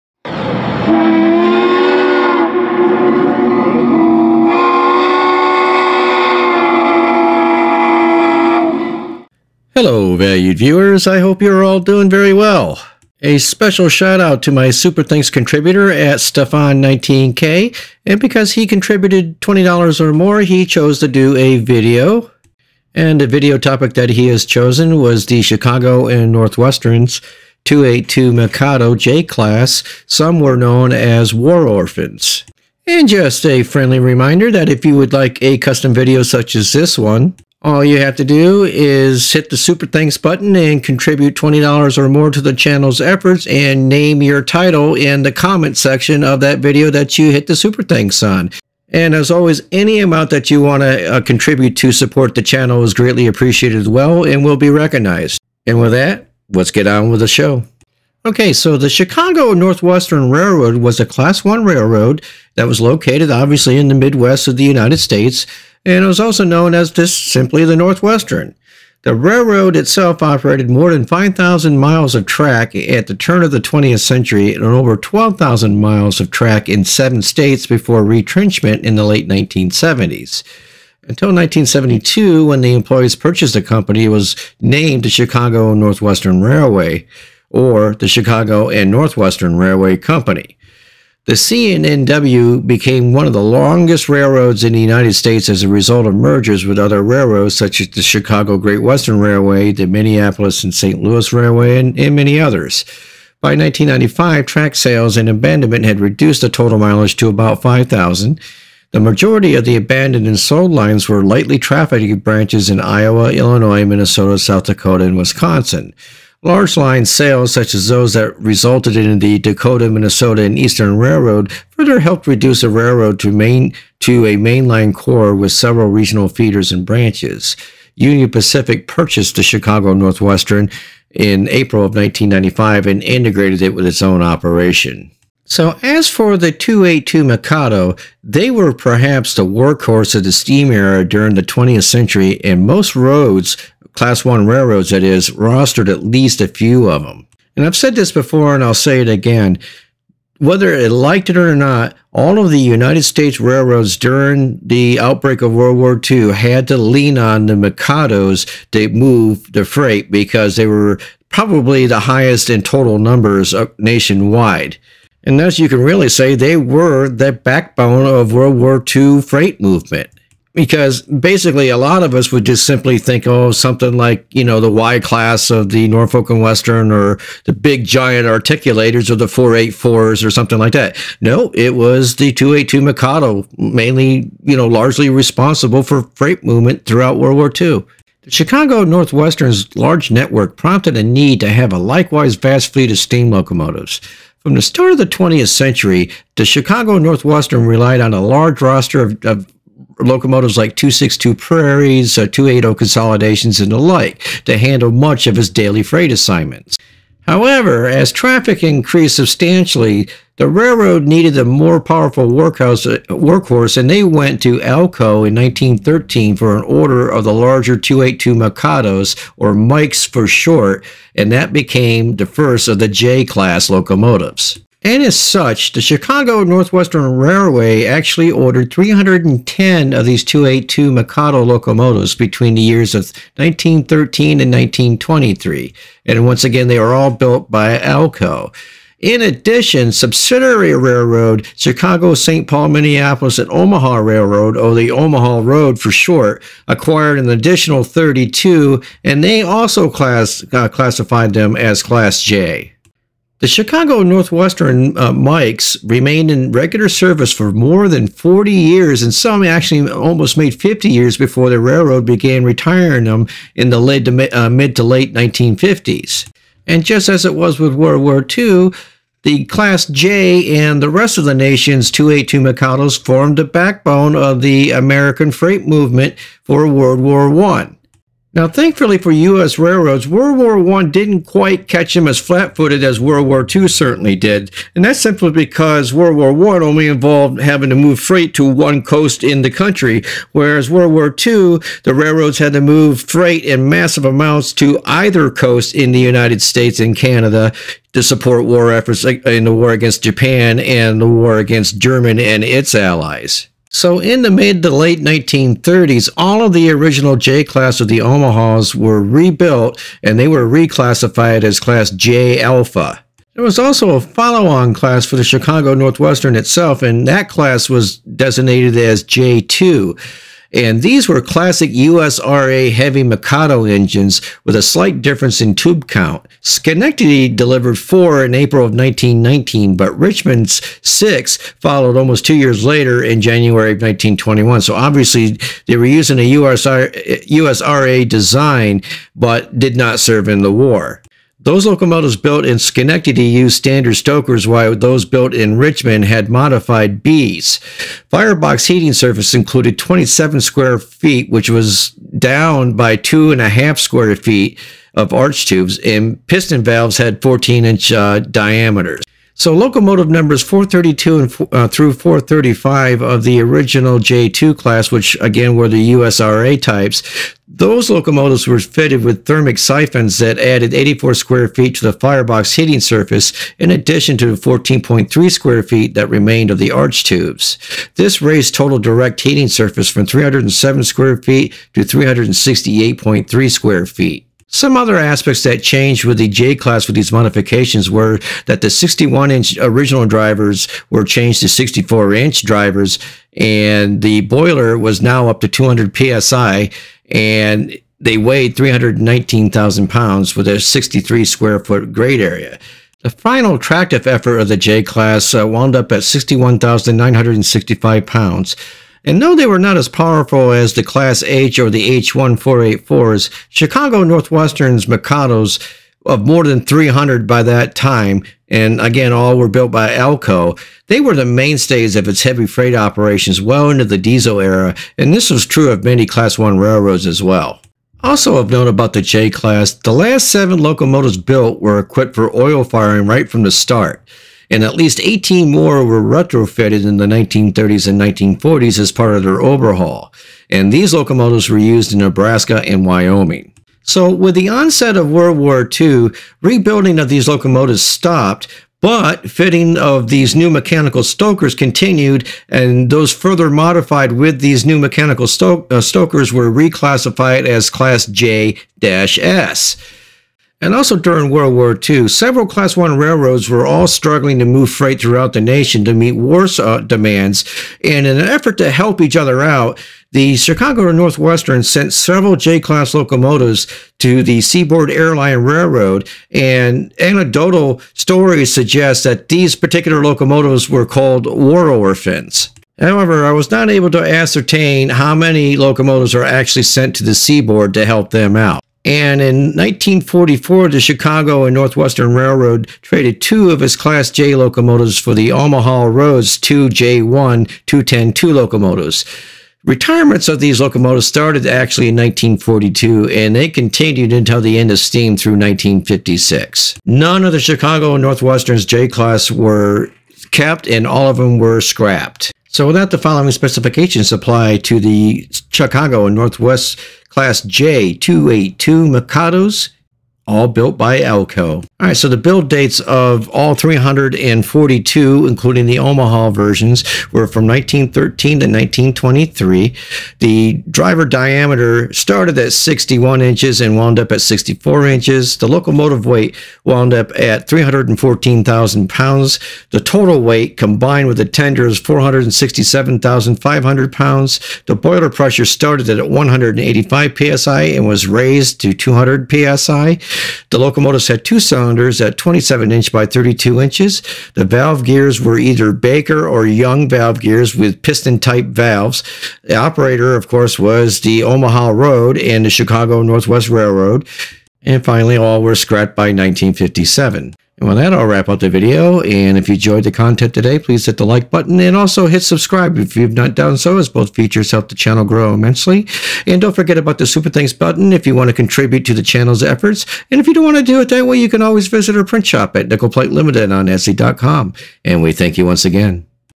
Chicago & NorthWestern J Class Locomotives
This video is a Super Thanks video that tells us about the Backbone of the Chicago & Northwestern Railroads Class J 2-8-2 Mikado Locomotives. Of which some where call "War Orphans" by an anecdotal account. These locomotives served the War efforts of BOTH World Wars. And remained in service for more than 4 decades.